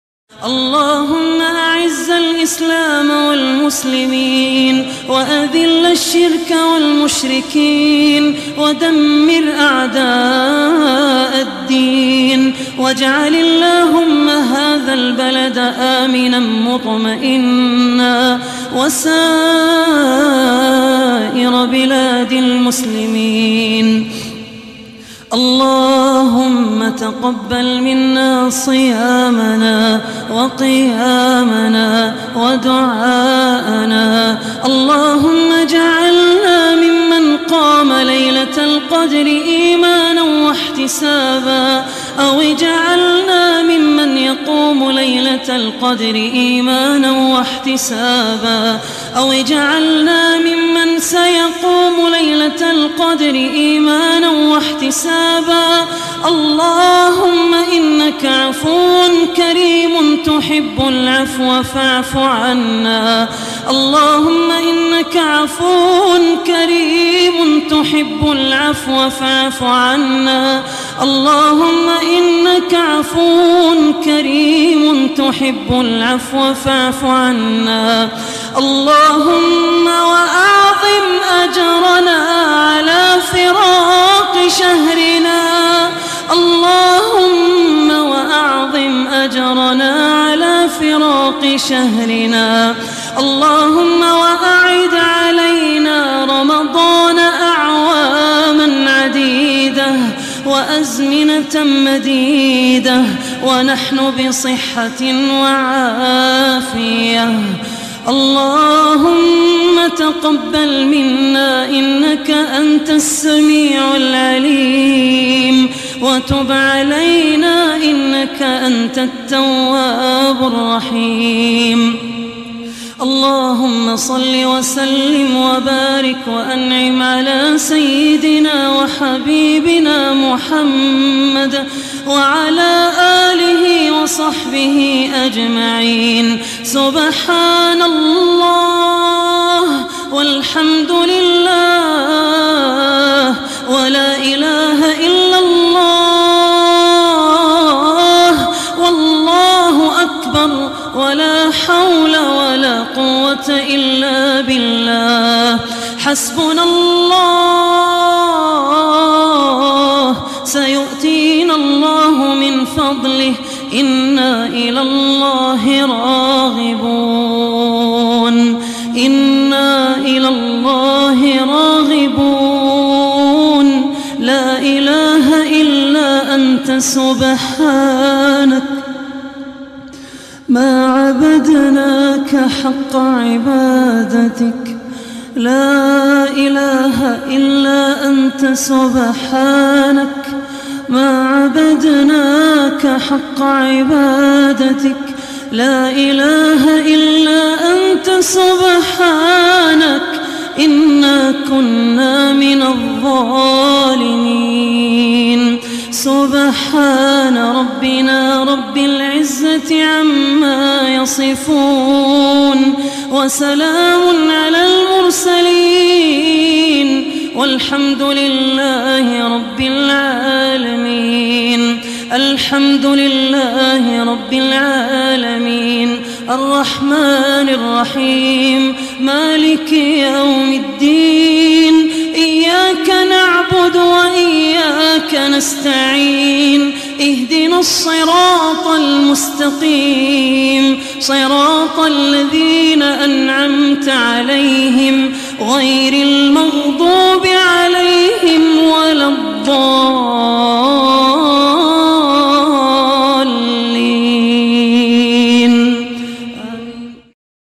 Dua🕋🕋🌜🌜🌜💕💕💕
Laylatul Qadr is a special night that holds great significance in Islam, and it is a time when a person can seek Allah’s mercy and blessings through acts of worship, including making sincere dua. By reciting the recommended dua with sincerity and humility, and by giving charity with a pure intention, a person can hope to gain the blessings and rewards of this auspicious night. May Allah grant us the ability to make the most of Laylatul Qadr and to seek His mercy and forgiveness through our supplications. Ameen.